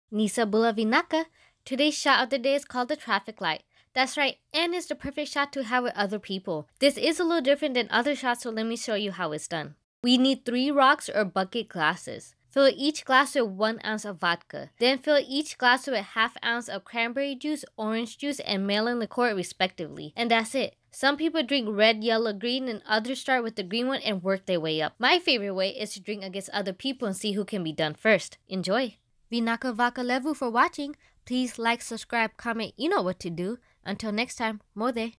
The Traffic Light Shot | fijian_dreamer
Bula! Tired of having the same ol' one shot at a time? Why not have three instead?! I love this shot because it's fun to go against other people & see who can finish first! PERFECT for ice-breakers, or just for getting the party started!
Please drink responsibly! Especially since this has 3 oz of alcohol combined!

Ingredients I Used:

Vodka: Absolut Vodka

Cranberry Juice: Ocean Spray Original Cranberry Juice Cocktail

Orange Juice: Simply Orange Pulp Free Orange Juice

Melon Liqueur: Midori Melon Liqueur
Like this shot?! Try these!

Please Note that I bought everything with my own money. All suggestions / opinions are my own.